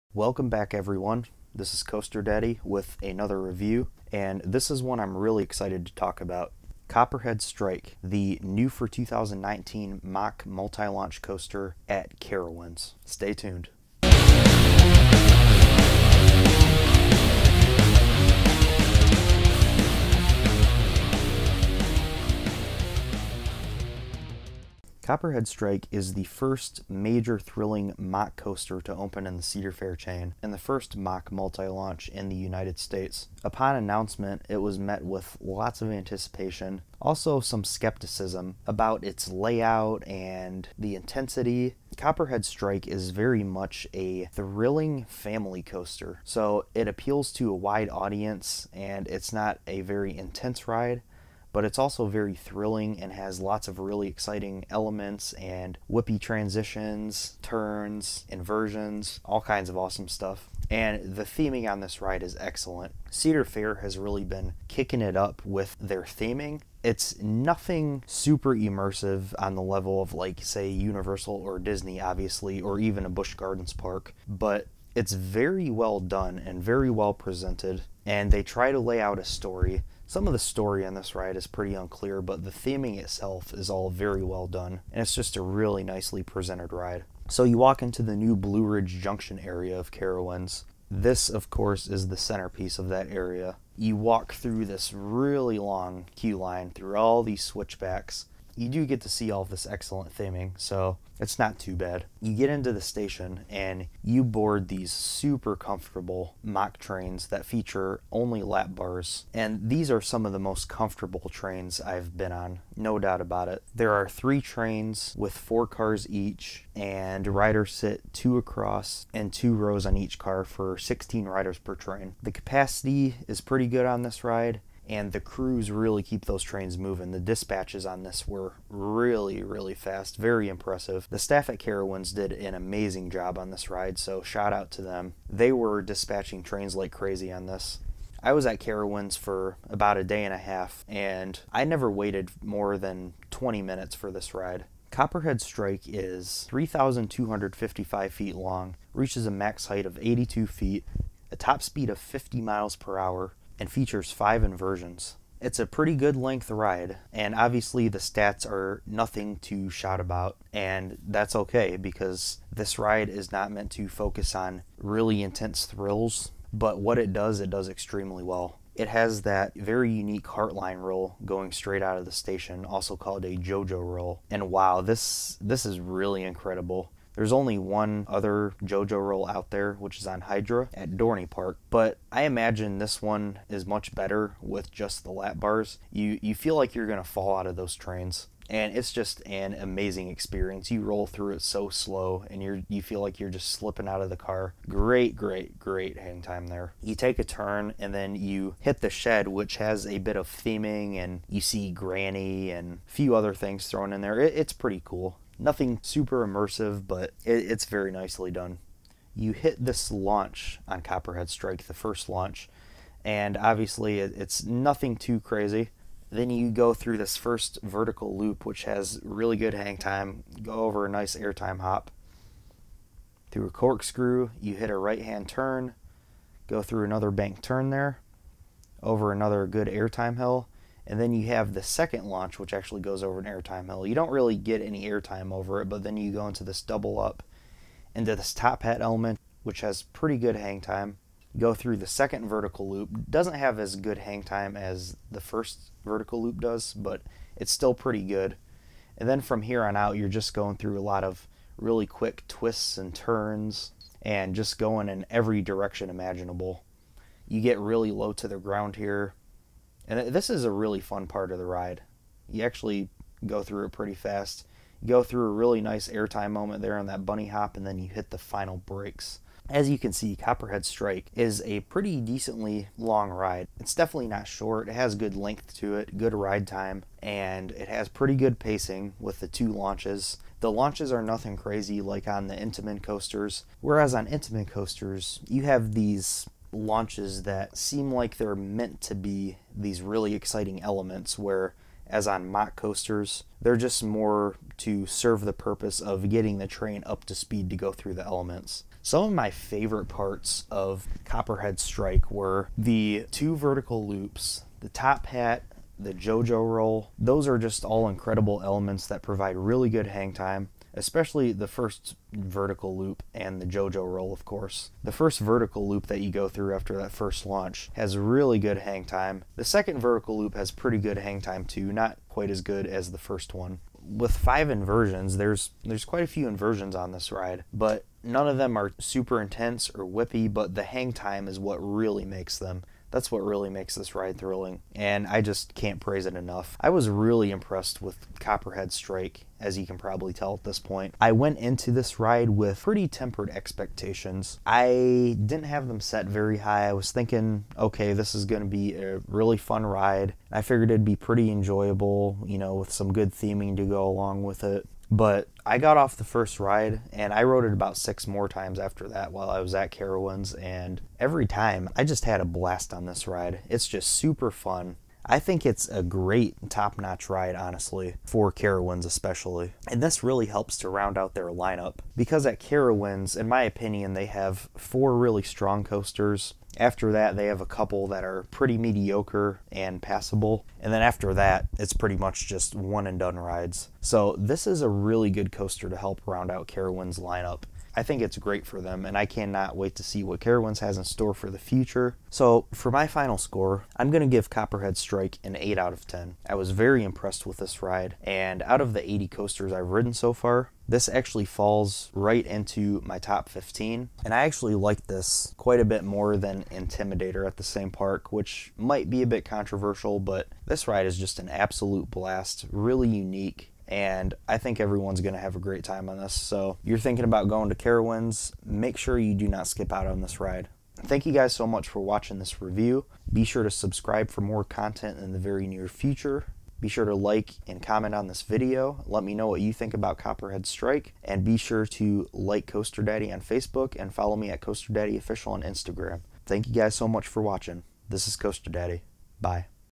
Copperhead Strike (Carowinds) Review Mack Multi-Launch Coaster
My official review of the brand new for 2019 Copperhead Strike at Charlotte, North Carolina's Carowinds!